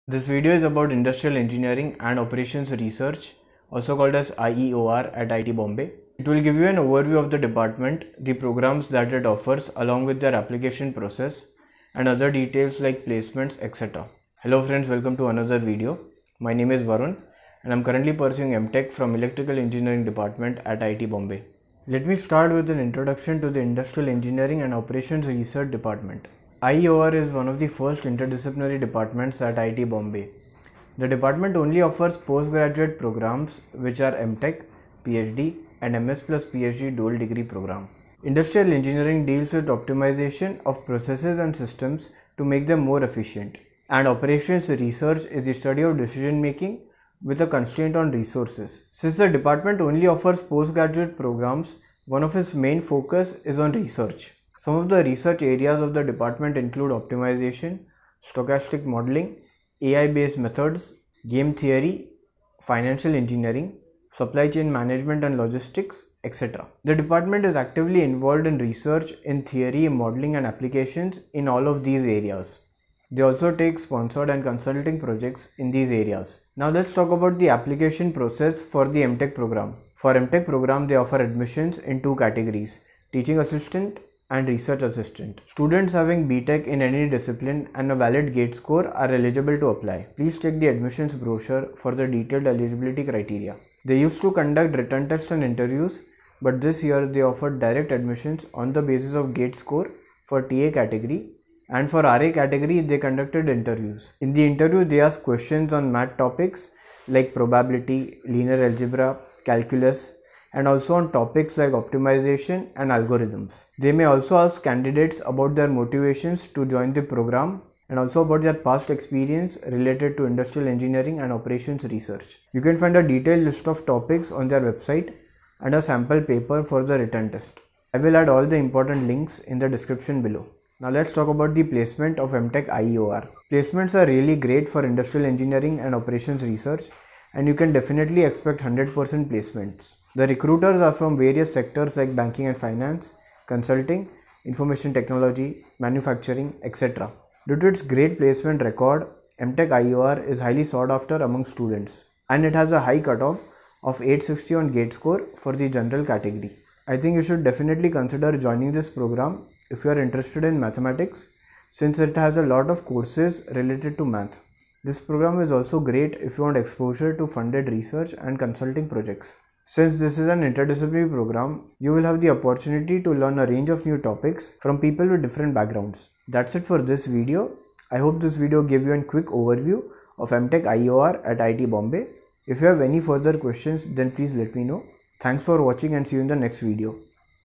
Mtech IEOR, IIT Bombay | Overview, Admission, Placements 🔥
This video is a quick overview of the Mtech IEOR program offered at IIT Bombay.

Chapters
00:00 Intro
00:22 IEOR Department, IIT Bombay
01:16 Mtech Admission Process
02:10 Placements & Final Points
02:56 Outro